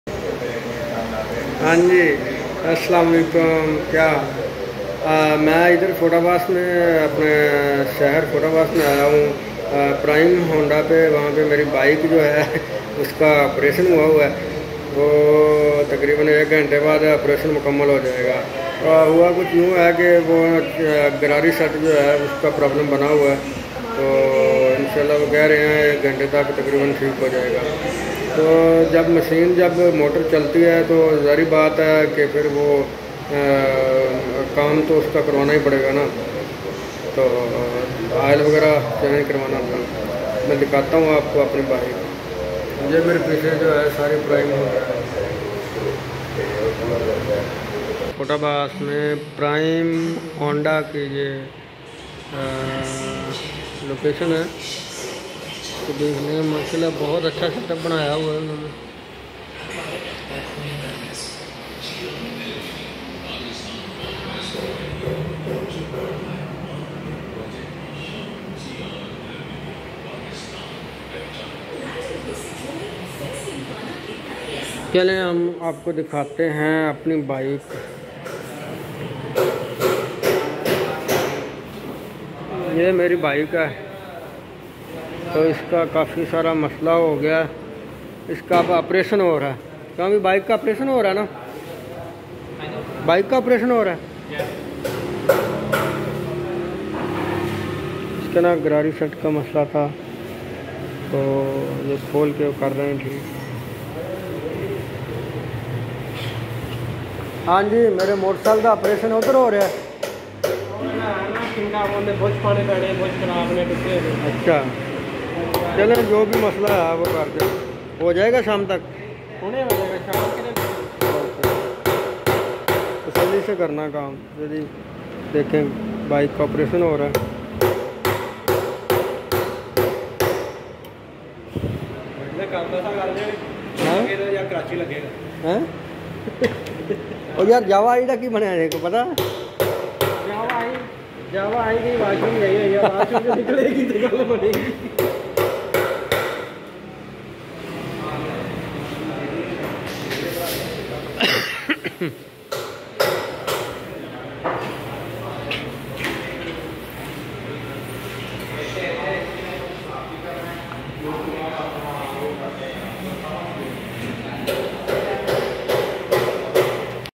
AJ MERI bike 🛵 Ka operation ho ra ||Prime Honda|| city fortabbas primehonda 17 April 2023
AoA
Hello 👋👋👋🤗🤗🤗
Everyone kese ho ap sab umeed krta hon khairat SE hon GE
Dosto ye ha Fortabbas Mera city.
yahan new showroom open howa .